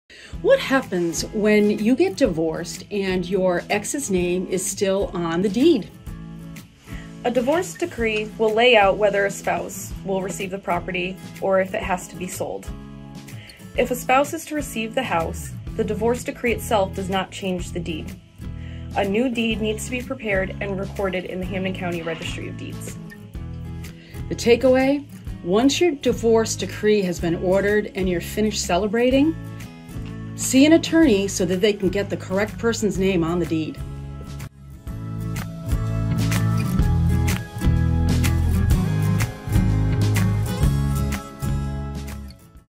Name on the Deed after Divorce
What Happens when you get divorced and your Ex's name is still on the Deed?